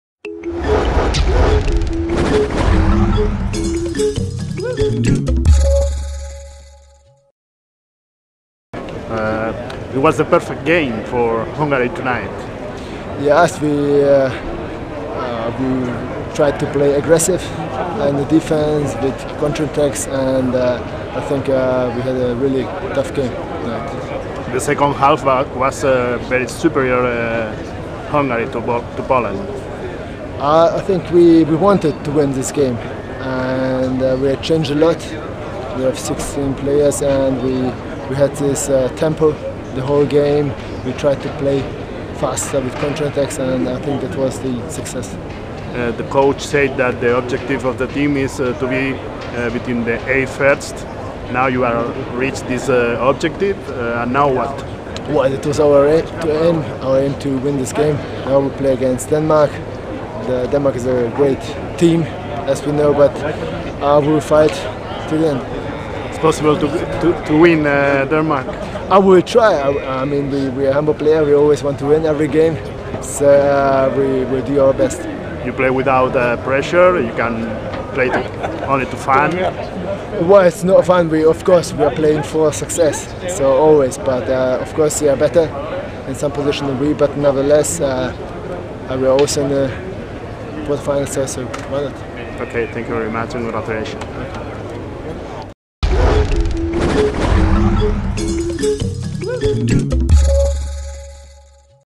MIXED ZONE: (Hungary-Poland) Tamas Mocsai
The Hungarian player Tamas Mocsai talked with Handball 2013 after his game versus Polannd in the eighth-finals.